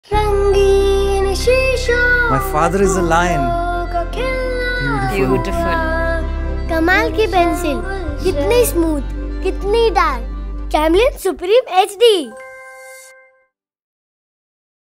Camlin Supreme HD Pencils for Smooth & Dark Writing | Kokuyo Camlin Ad
With Camlin HD Supreme Pencils, experience writing that flows as smoothly as your thoughts. In our new product film, see how Rohan’s heartfelt message to his dad brings smiles to his parents, as these pencils make every word special and memorable.

Excited to verbalise your thoughts and effortlessly pen them down? Tap to explore Camlin kids’ product range!